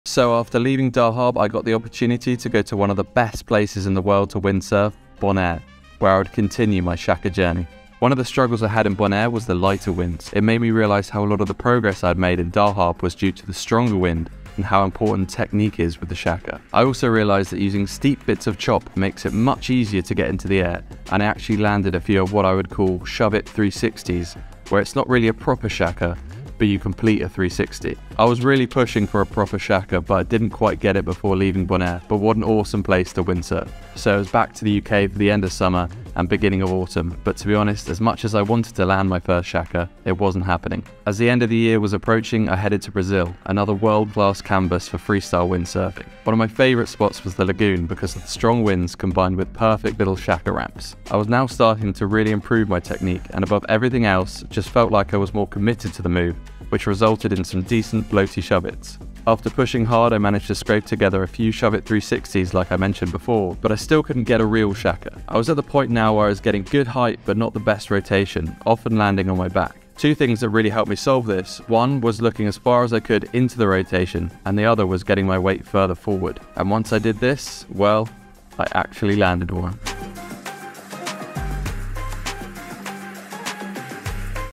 I finally landed it!
Another episode in the Road to Shaka series. A windsurfing series where I document my progress of learning new moves. Which moves would you like to see me try?